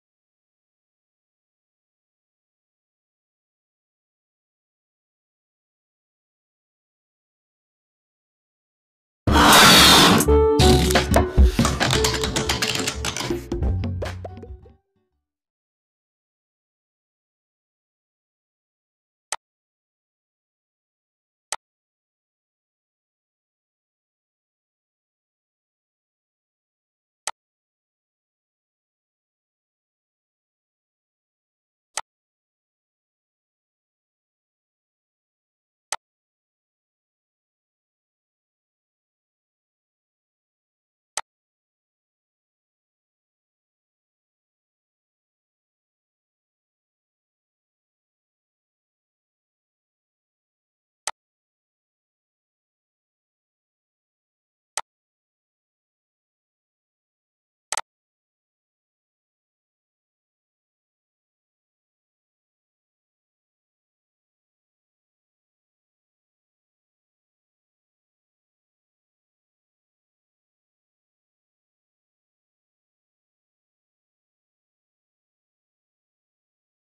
How to apply bubbles screen saver in pc/laptop
I told you in this video how can you apply bubbles screen saver in your computer.